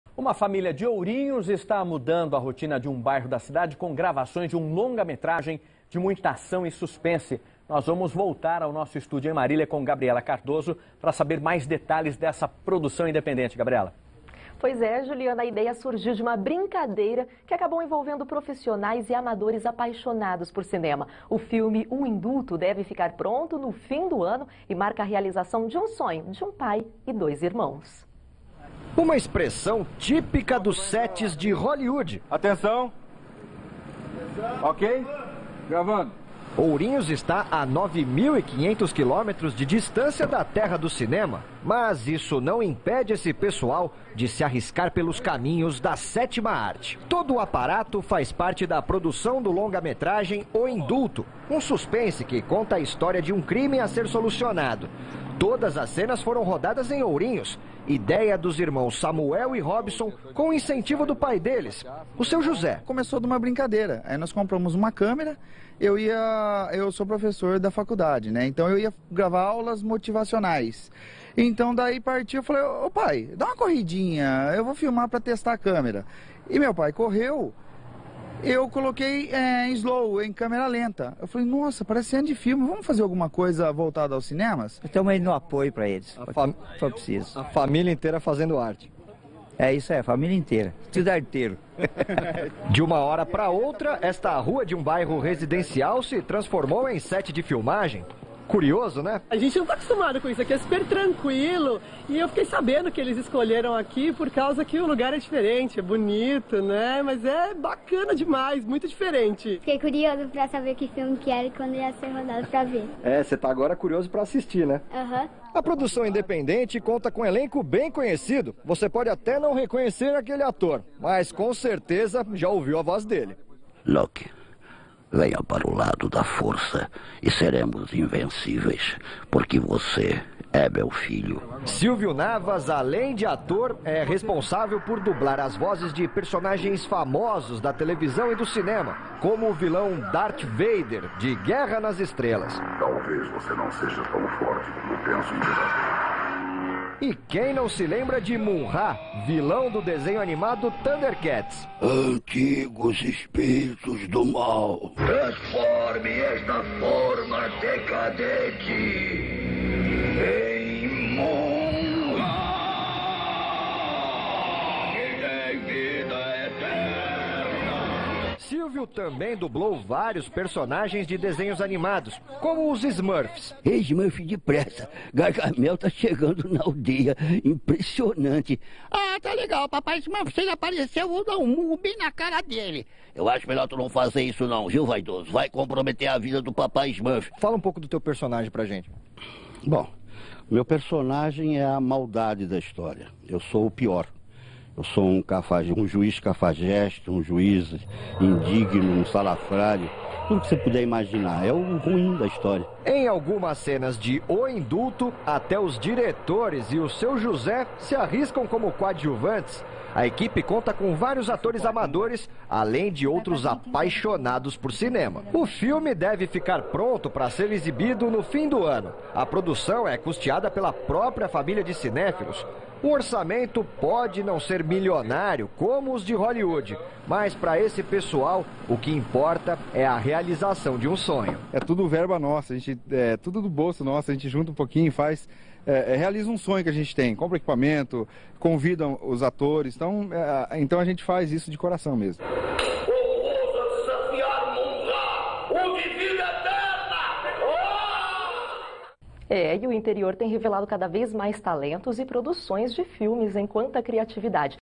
O Indulto - Matéria Rede Globo TV Tem Bauru Alexandre Azank Longa Metragem de Ourinhos
Matéria exibida pela Rede Globo TV Tem Bauru, Alexandre Azank mostra bastidores de uma cena com Silvio Navas, João Gambini, Milton Rigonati, Jéssica, André Duarte, Eduardo Machado, Robson Sanches, José Roberto Mathiasso, Samuel Sanches - San Filmes